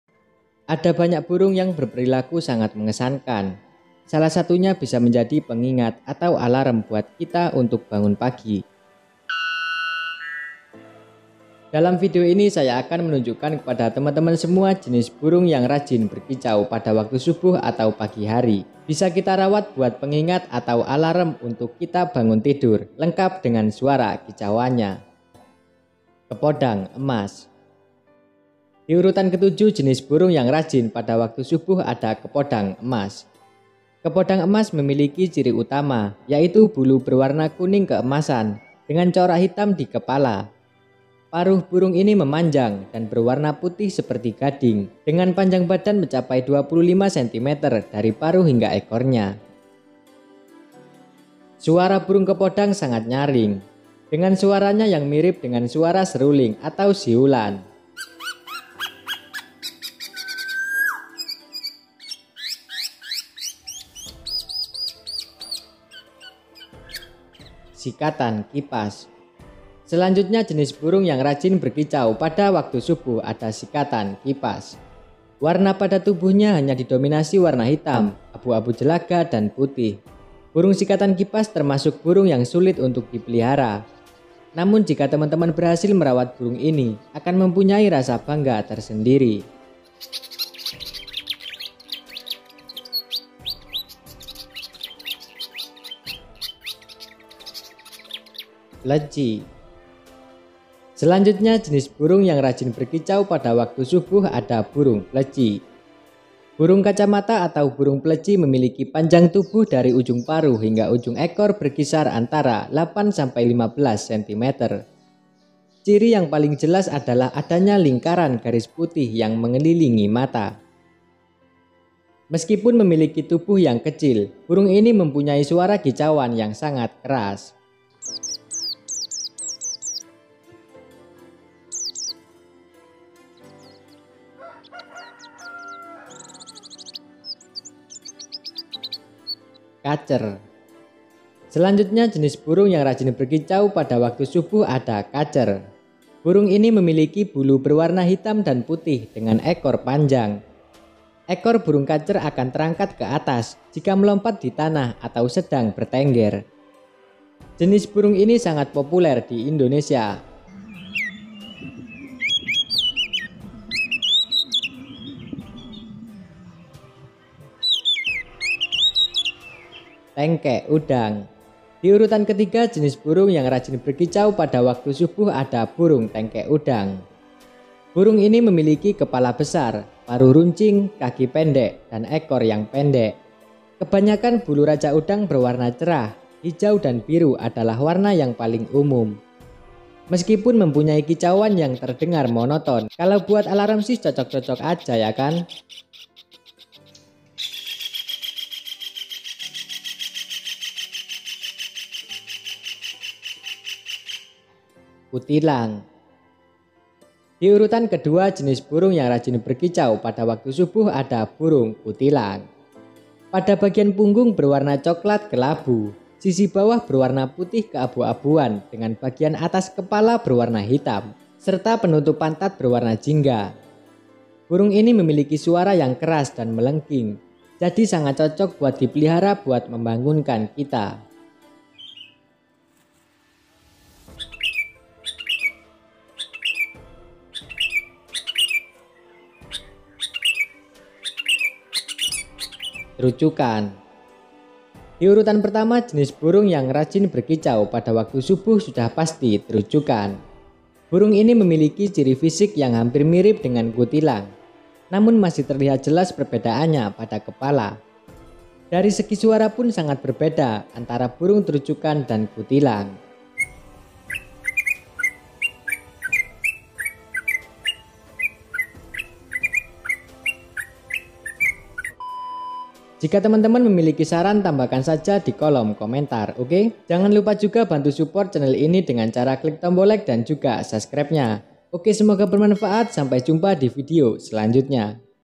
7 JENIS BURUNG RAJIN KICAU WAKTU SUBUH
Jenis – jenis burung yang rajin berkicau saat subuh, cocok buat alarm untuk bangun pagi
Apakah teman-teman mengenal salah satu burung dalam video ini? Apakah Teman-teman mengetahui spesies yang memiliki lagu yang aneh? Jika ya, silakan berkomentar!